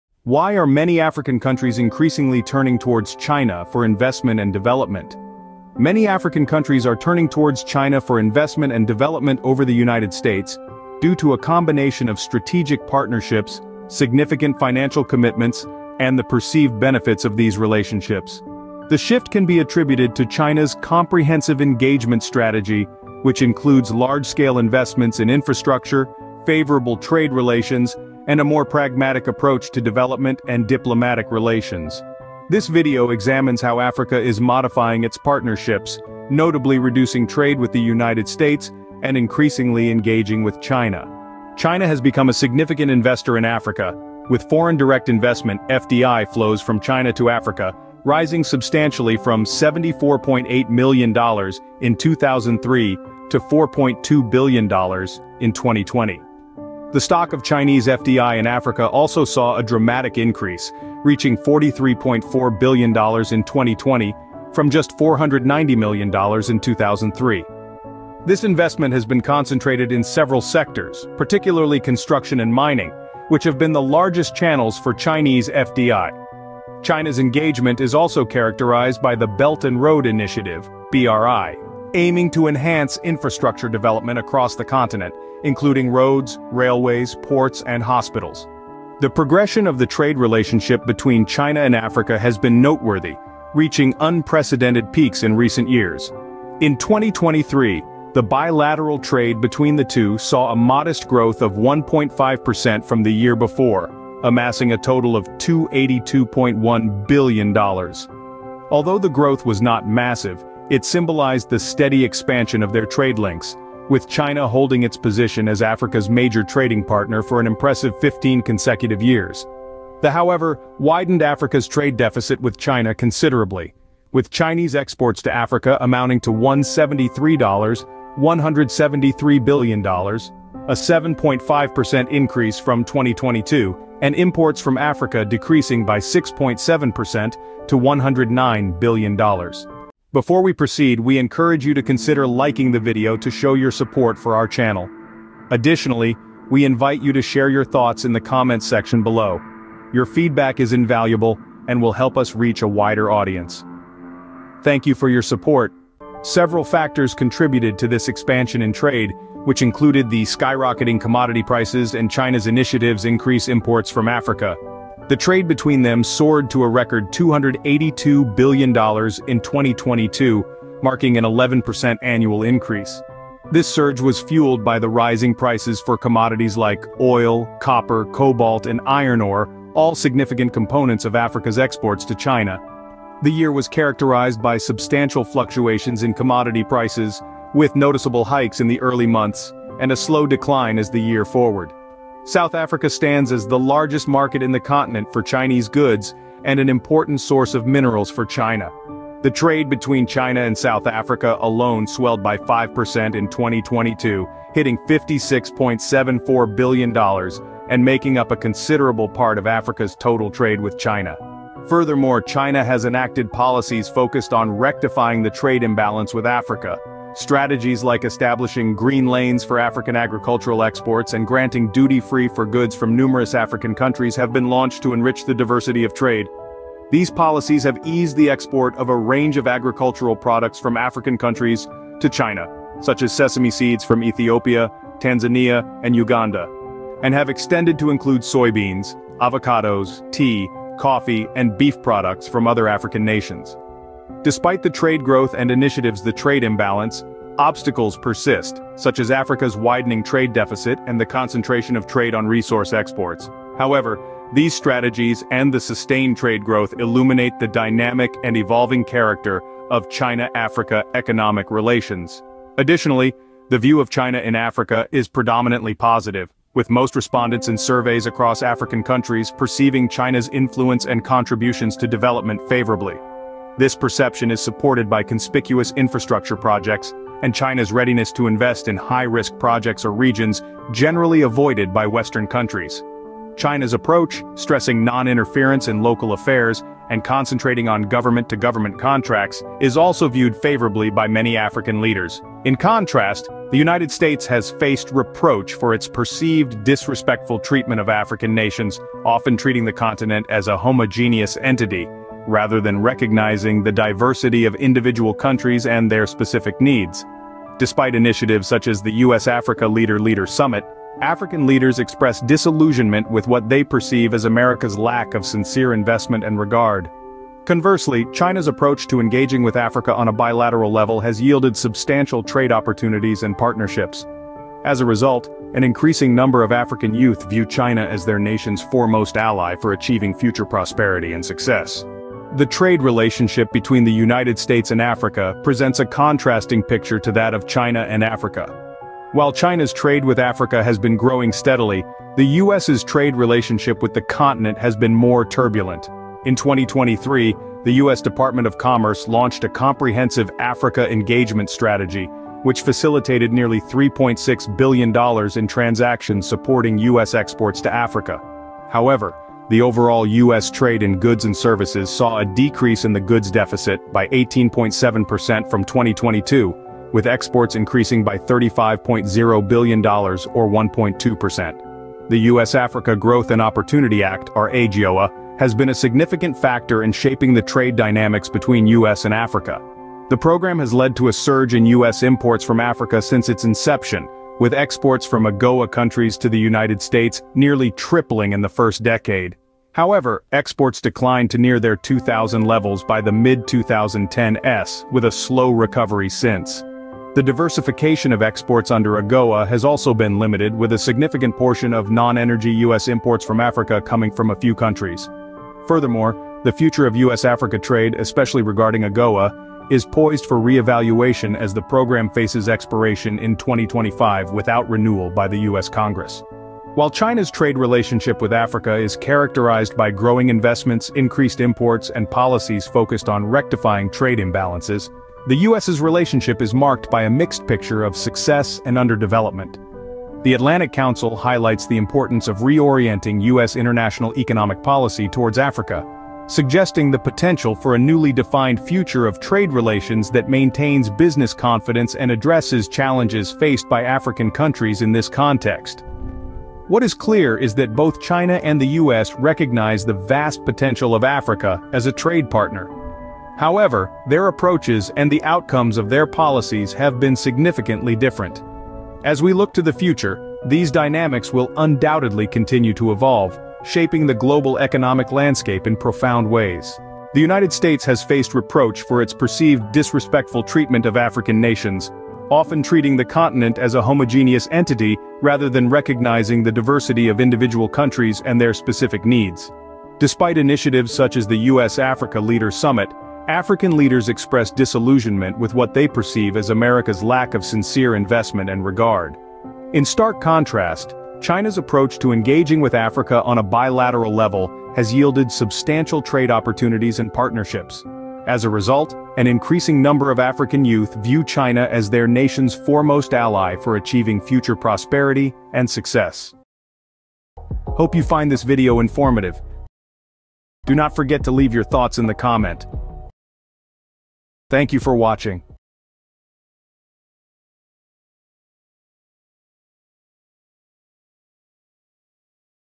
China Overtake Us in Africa: Why Africa Turning toward China?
why are many African countries increasingly turning towards China for investment and development?
Many African countries are increasingly turning towards China for investment and development over the United States due to a combination of strategic partnerships, significant financial commitments, and the perceived benefits of these relationships. The shift can be attributed to China's comprehensive engagement strategy, which includes large-scale investments in infrastructure, favorable trade relations, and a more pragmatic approach to development and diplomatic relations.

This video examines how Africa is modifying its partnerships, notably reducing trade with the United States and increasingly engaging with China.